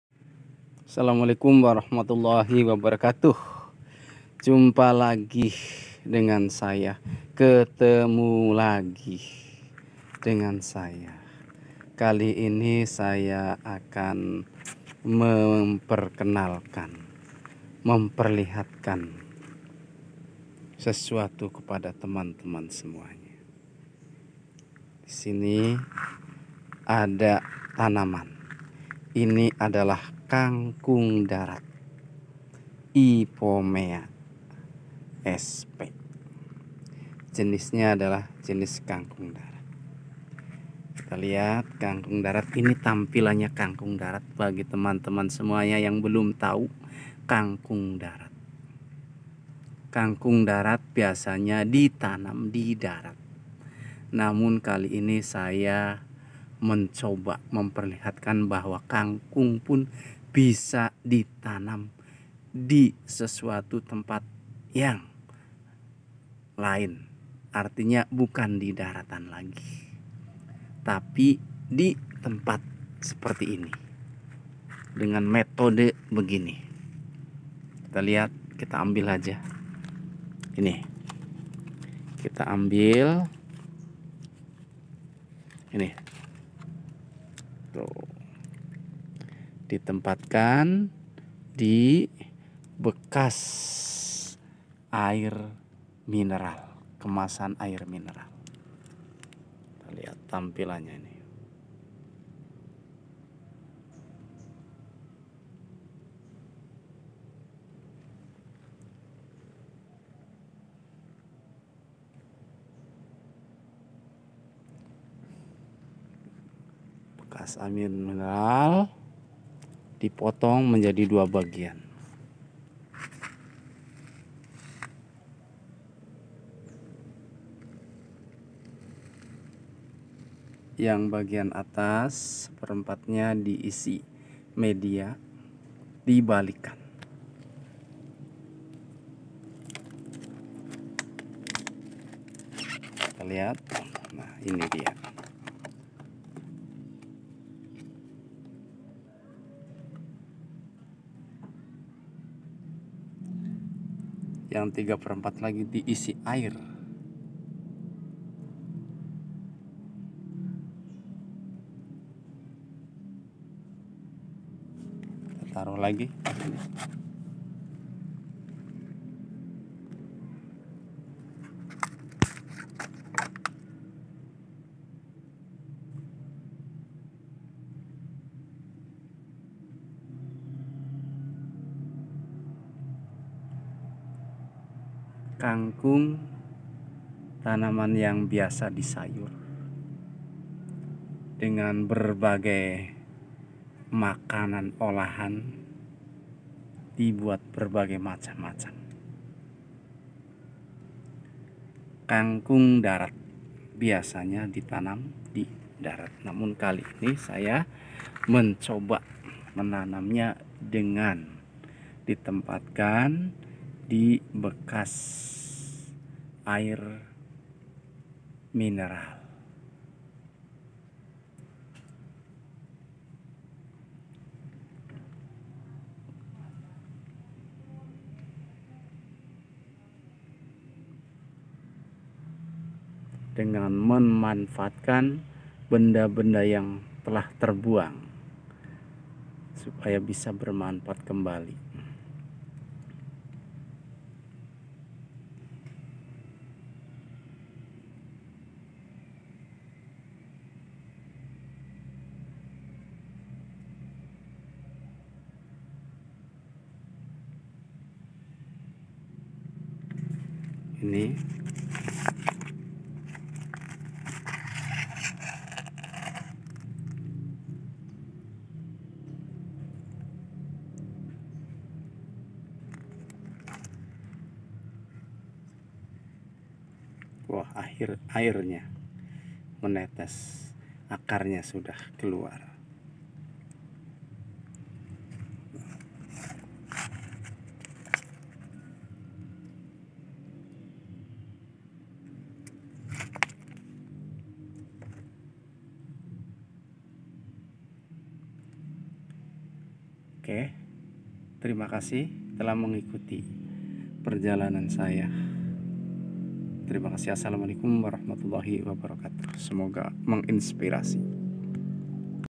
MENANAM KANGKUNG DI BOTOL BEKAS
menanam kangkung di botol bekas air mineral kemasan.